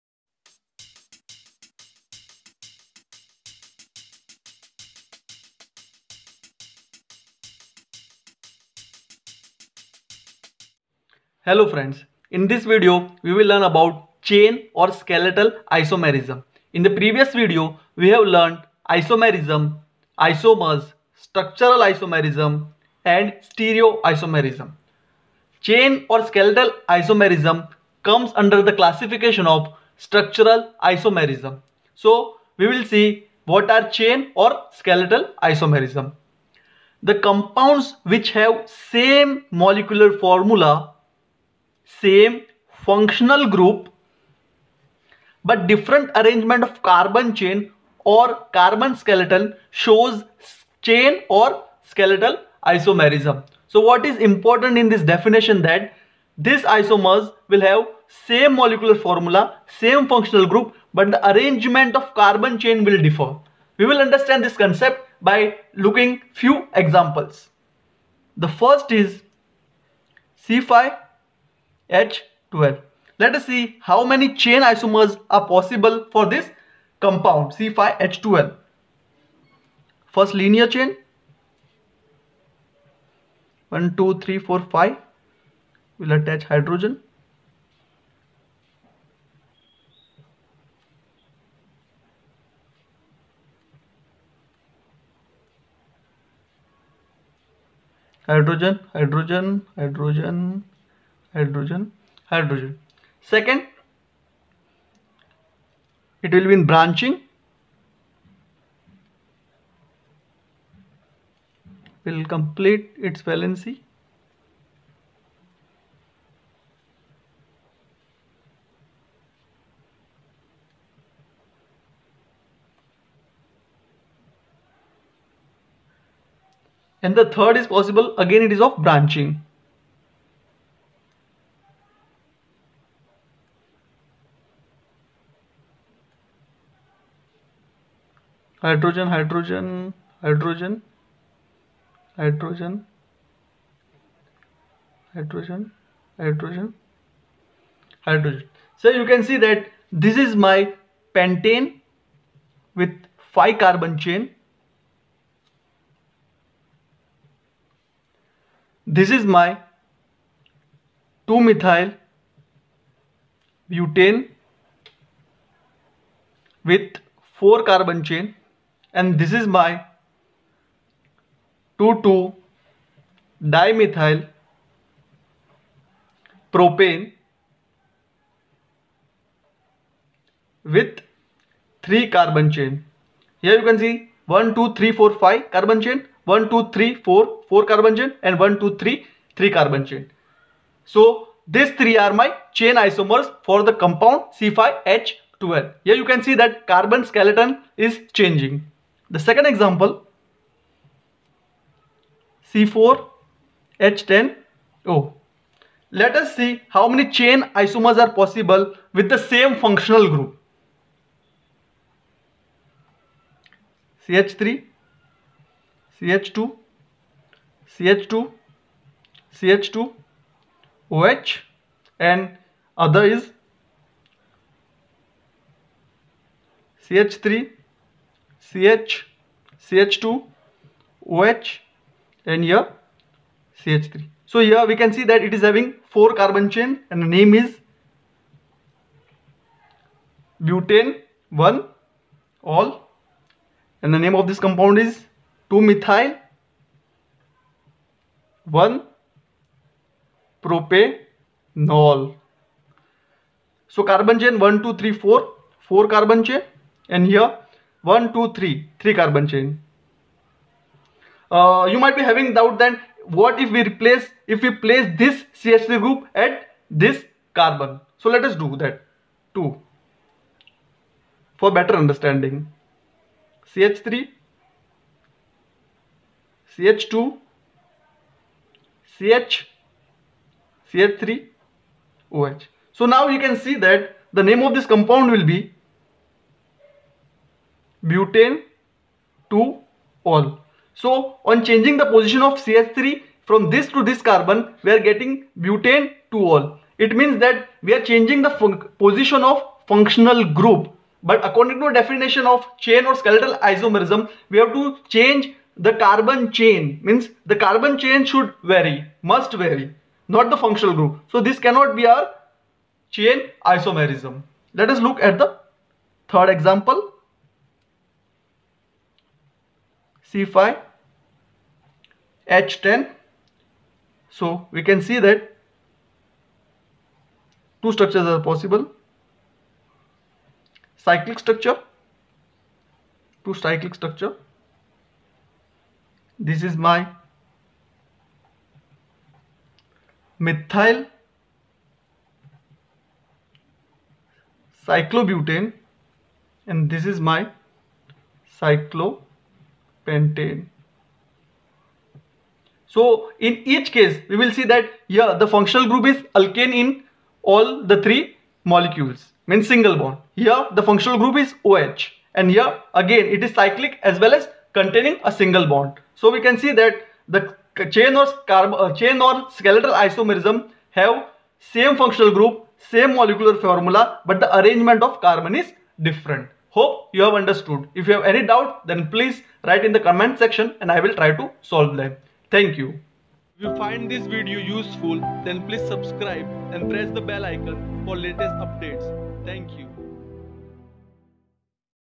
Chain/Skeletal Isomerism | Isomerism - Part-2
This video explains the concept of Chain/Skeletal Isomerism along with examples.